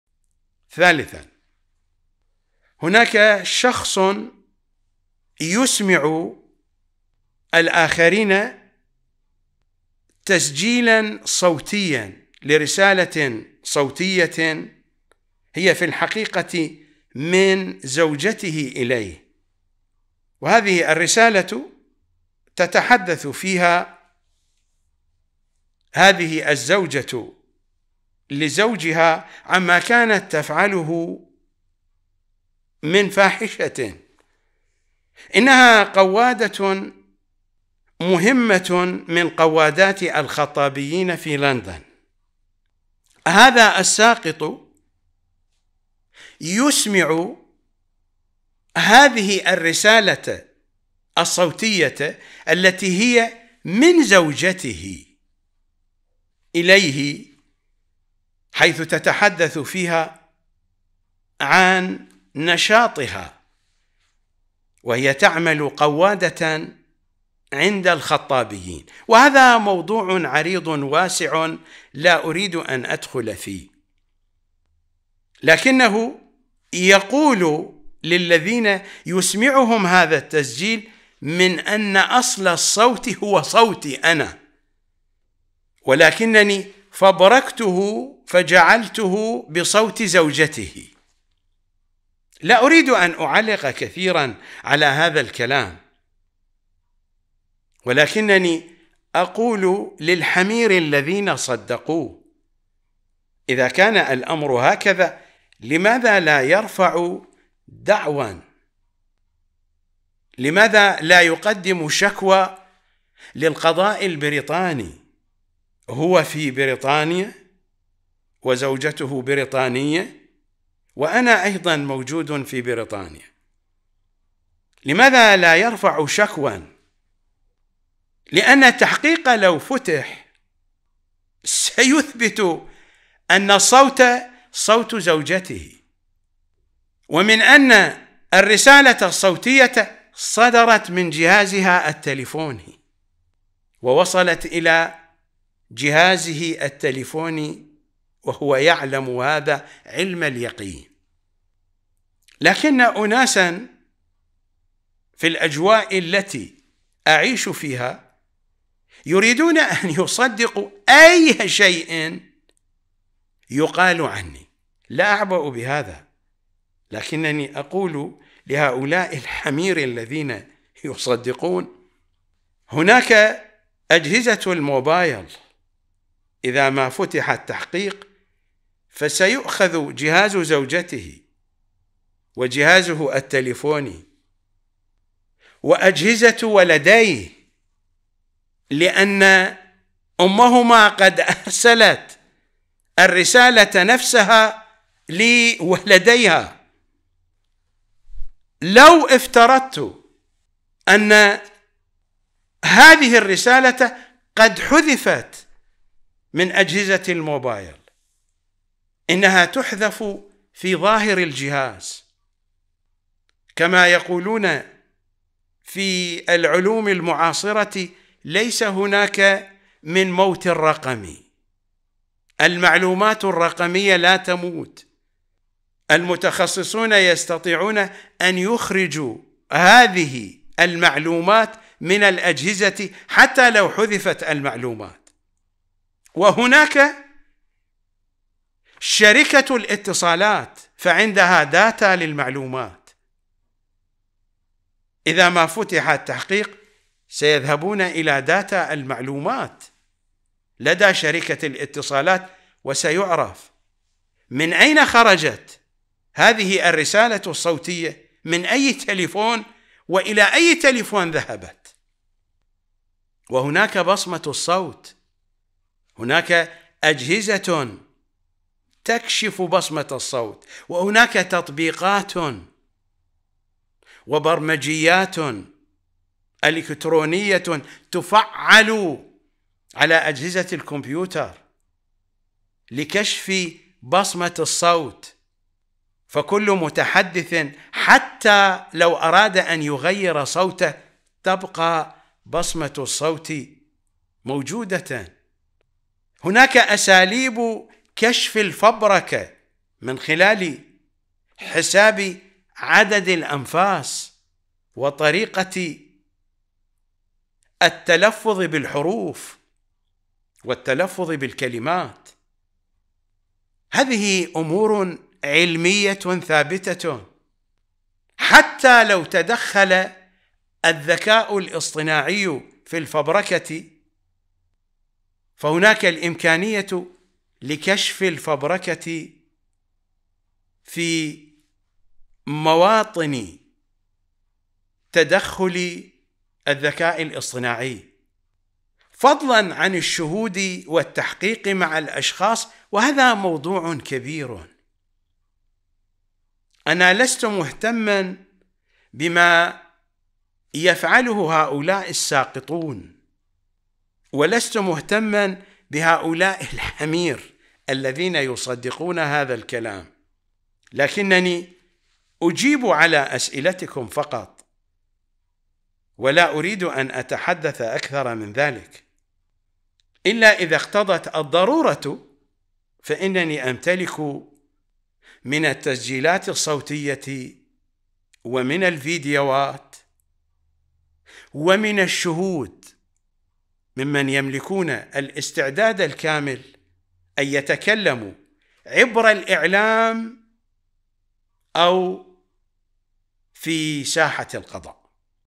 هؤلاء هم اعداء القمر الفضائية والاستاذ الغزي ..! هذا حالهم (قواديين وسكًاطات) والمراجع خير دليل ...
يا زهراء
  دردشة مع الزهرائيين فقط - ليالي شعبان 1445هـ